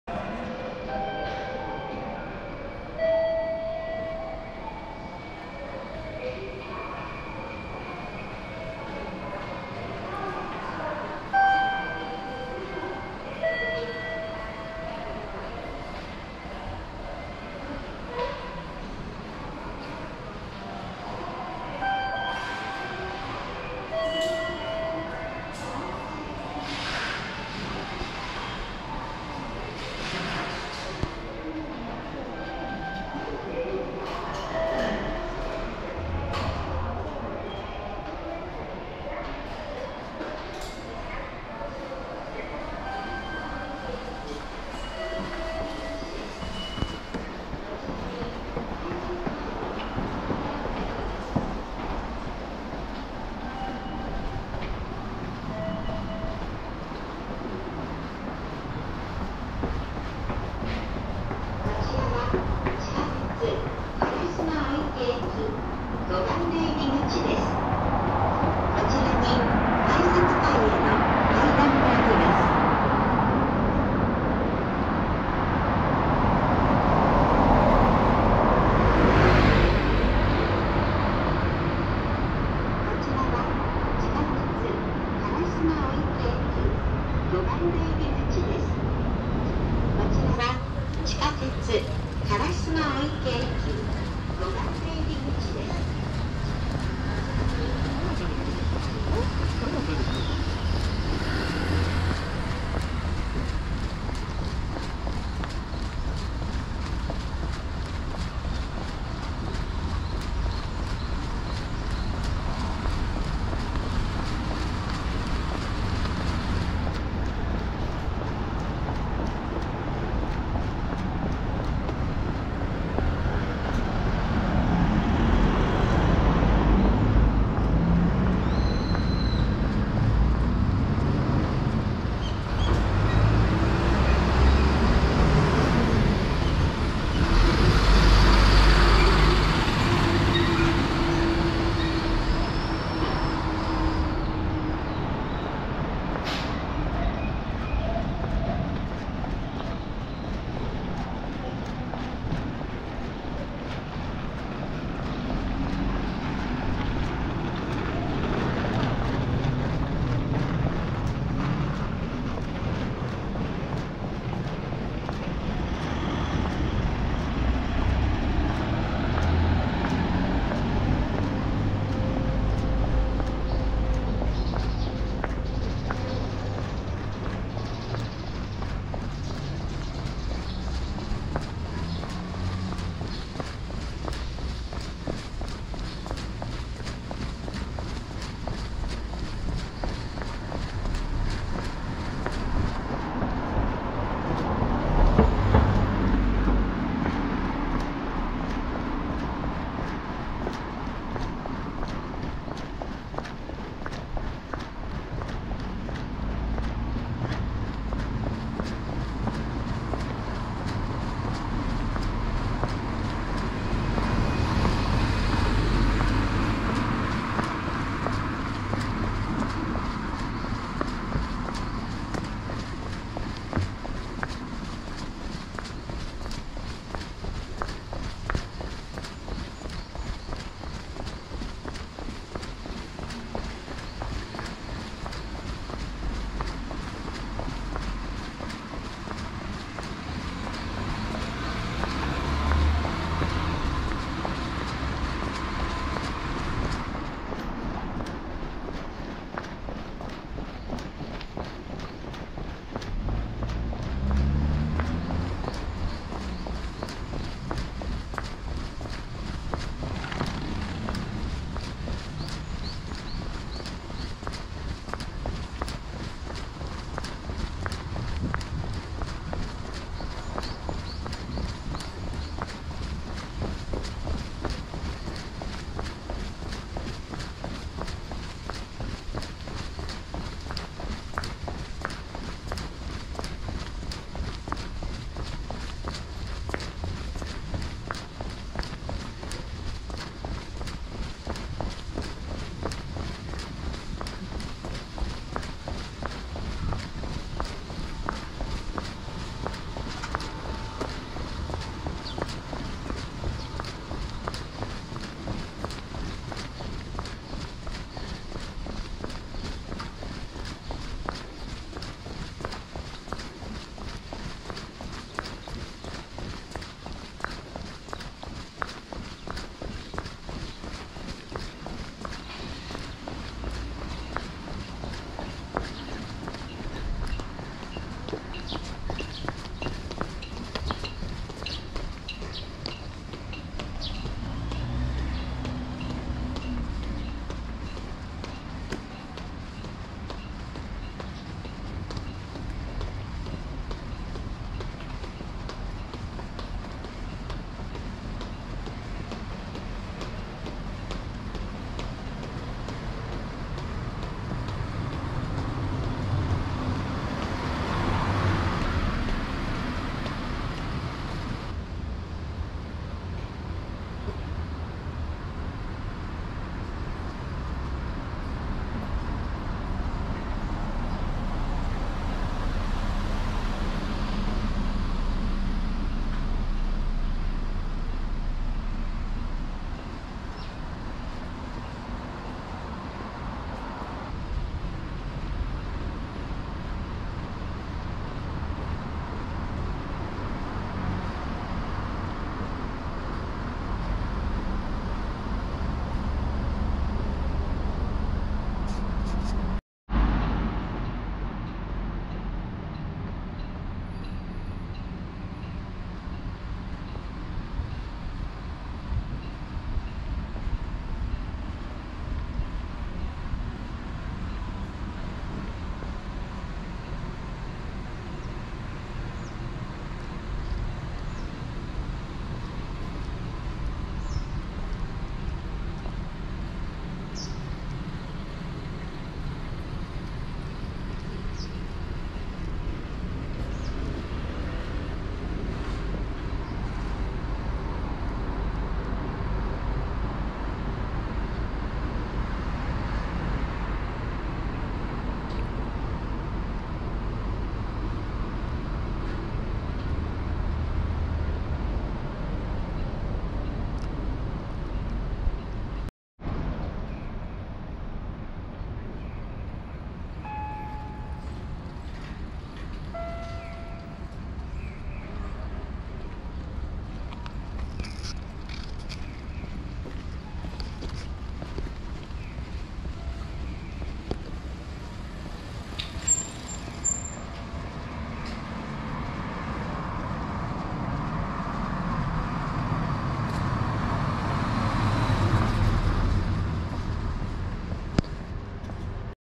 【京都】烏丸御池駅からミシュラン5つ星＋「俵屋旅館」& 5つ星「柊家（ひいらぎや）」まで Run and guide
烏丸御池駅から俵屋旅館と柊家（ひいらぎや）まで走ってご案内いたします。俵屋旅館は、スティーブ・ジョブズ氏が好んで泊まっていたと言われており、1700年代初期に創業した歴史のある宿です。俵屋旅館の目の前には、柊家（ひいらぎや）という1818年創業の老舗旅館があります。烏丸御池駅からエースホテルのある新風館を走り抜けます。目的地の二つの宿は、2022年とは思えない風情を感じさせてくれます。